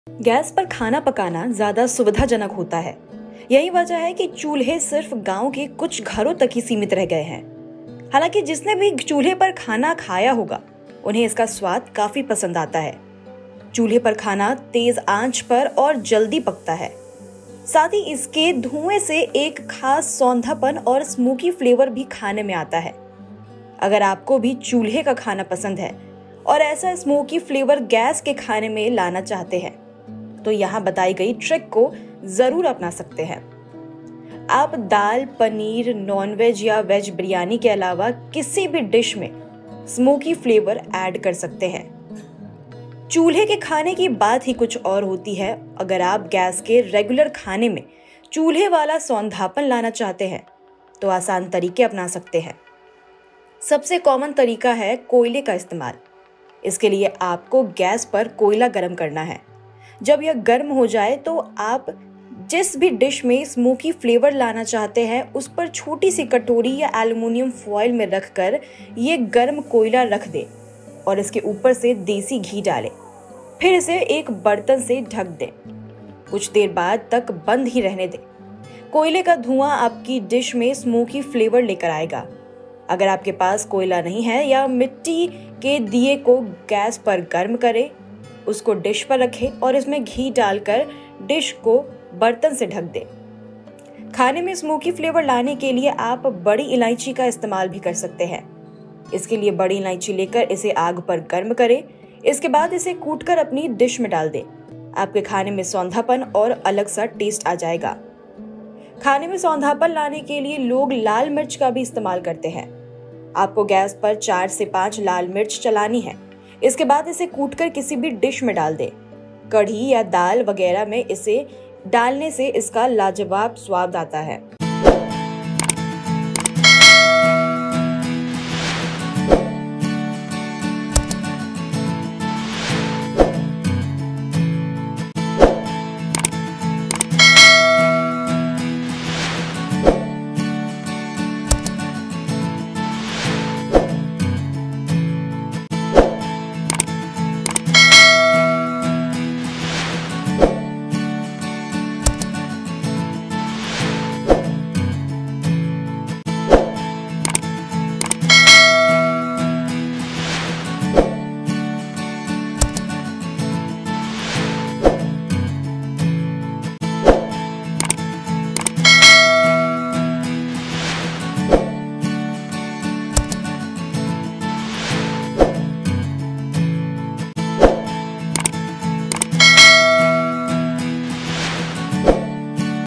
खाने में लाए मिट्टी के चूल्हे सा स्वाद और सौंधापन, अपनाएं ये ट्रिक | NEWS PR
खाने में लाए कोयले का फ़्लेवर और सौंधापन, अपनाएं ये ट्रिक | NEWS PR